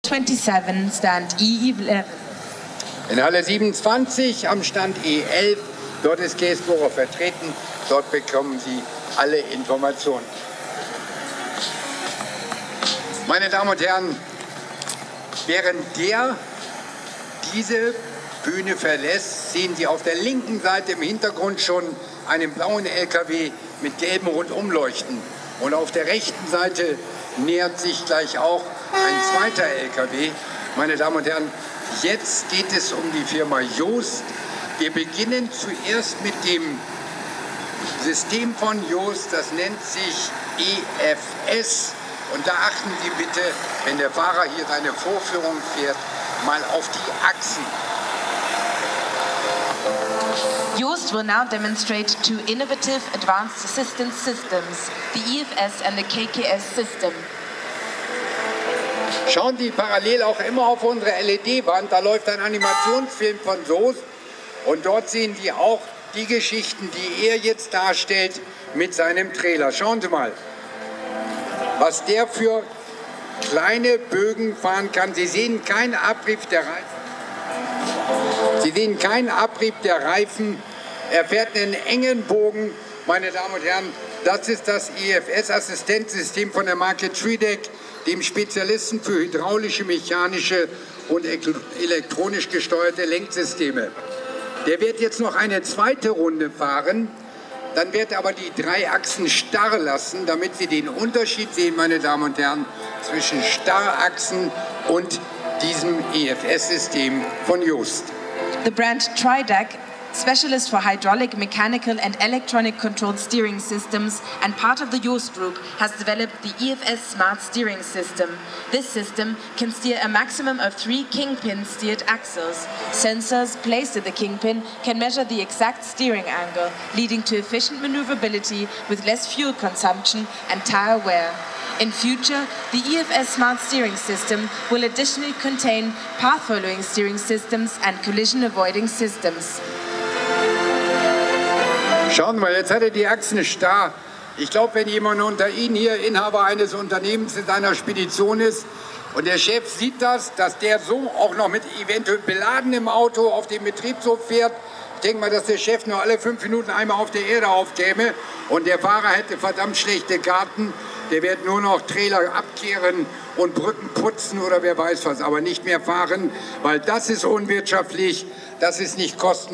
IAA Nutzfahrzeuge 2016 Hannover IVECO Kässbohrer DAF Jost
Auf der IAA Nutzfahrzeuge 2016 konnte ich diesen IVECO-Stralis mit Kässbohrer-Sattelauflieger sowie diesen DAF filmen, der ebenfalls einen Sattelauflieger zog. Er hatte das EFS von Jost auf der Demofläche vorgeführt.